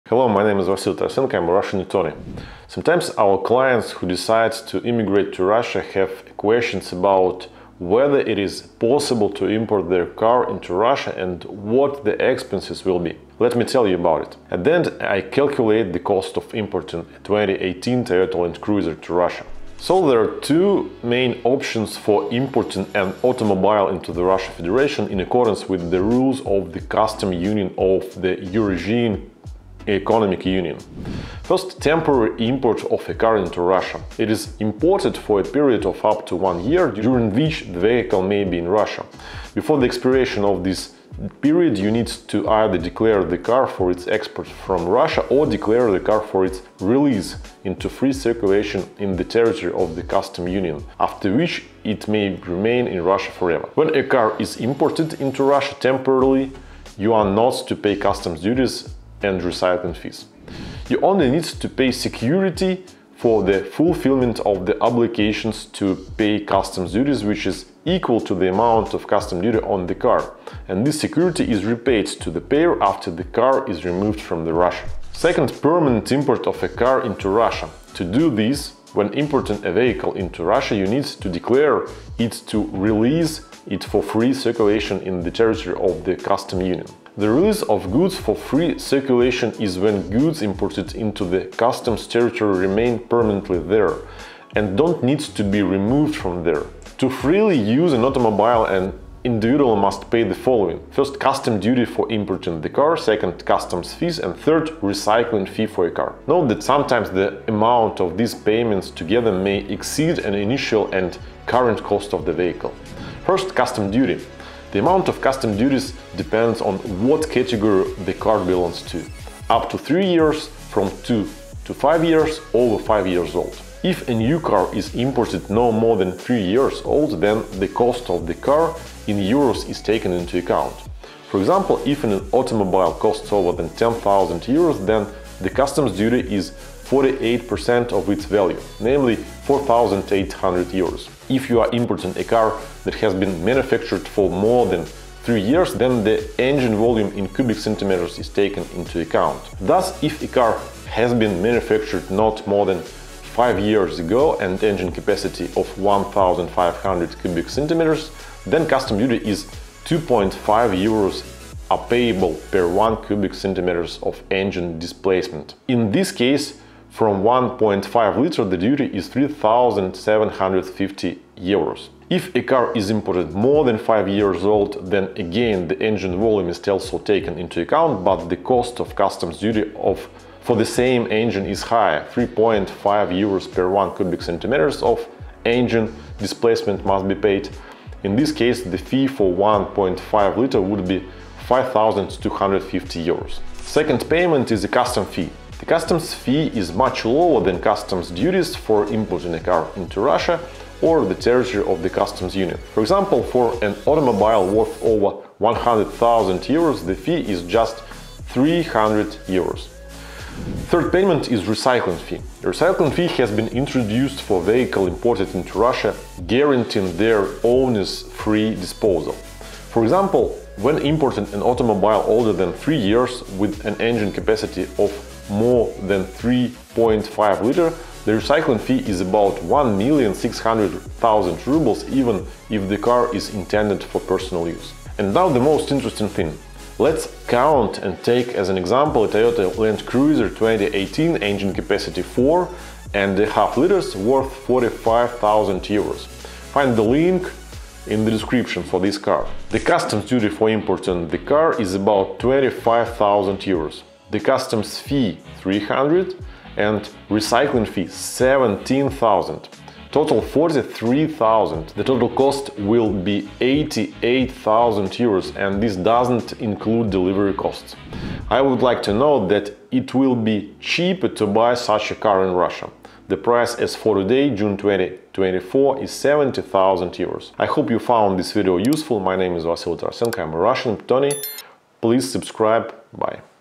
HOW TO IMPORT A CAR INTO RUSSIA?
0:00 Intro
0:20 Temporary or permanent import of a car
1:37 Payments if you import your car into Russia forever
3:55 Calculation example

There are 2 OPTIONS for importing an automobile into the Russian Federation in accordance with the rules of the Customs Union of the Eurasian Economic Union:

1. TEMPORARILY. It is imported for a period of up to one year, during which the vehicle may be in Russia. Before the expiration of this period, you need to either declare the car for its export from Russia, or declare the car for its release into free circulation in the territory of the Customs Union (after which it may remain in Russia forever).
When a car is imported into Russia temporarily, you are not to pay customs duties and recycling fees.
You only need to pay security for the fulfillment of the obligation to pay customs duties, which is equal to the amount of customs duty on the car. And this security is repaid to the payer after the car is removed from Russia.

2. PERMANENTLY . To do this, when importing a vehicle into Russia, you need to declare it to release it for free circulation in the territory of the Customs Union. The release of goods for free circulation is when goods imported into the customs territory remain permanently there and do not need to be removed from there.

To freely use an automobile, an individual must pay:
1. customs duty for importing the car,
2. customs fee,
3. recycling fee for a car.
Note that sometimes the amount of these payments may exceed the initial and current cost of the vehicle.

1. CUSTOMS DUTY
The amount of customs duties depends on what category the car belongs to.
- Up to 3 years old
- From 3 to 5 years
- Over 5 years old

If a new car is imported, no more than 3 years old, then the COST OF THE CAR is taken into account.
For example, if an automobile costs over €10,000, then the customs duty is 48% of its value, namely €4800.

If you are importing a car that has been manufactured for more than 3 years, then the ENGINE VOLUME is taken into account.
Thus, if a car has been manufactured not more than 5 years ago and engine capacity of 1500 cubic centimeters, then custom duty is €2.5 are payable per 1 cm2 of engine displacement. In this case, from 1.5 liter the duty is three thousand seven hundred fifty euros.

If a car is imported, more than 5 years old, then, again, the ENGINE VOLUME is also taken into account, but the cost of customs duty for the same engine is higher - 3.5 euros per 1 cubic centimeter of engine displacement must be paid. In this case, the fee for 1,5 l would be 5250 euros.

2. CUSTOMS FEE
The customs fee is much lower than the customs duties for importing a car into Russia or the territory of the Customs Union.
For example, for an automobile worth over 100 thousand euros, the fee is €300.

3. RECYCLING FEE
A recycling fee has been introduced for vehicles imported into Russia, guaranteeing their owners free disposal. For example, when importing an automobile older than 3 years with an engine capacity of more than 3.5 l, the recycling fee is about 1,600,000 rubles, even if the car is intended for personal use.

For example, a Toyota Land Cruiser, 2018, engine capacity 4.5 liters, worth 45,000 EUR

1. The customs duty for importing the car is about 25,000 euros
2. The customs fee, 300
3. recycling fee: 17,000
Total: 43,000 EUR
The total cost will be 88 thousand euros; and this does not include delivery costs!

I would like to note that it will be cheaper to buy such a car in Russia: the price as of today, June 2024, is 70,000 euros.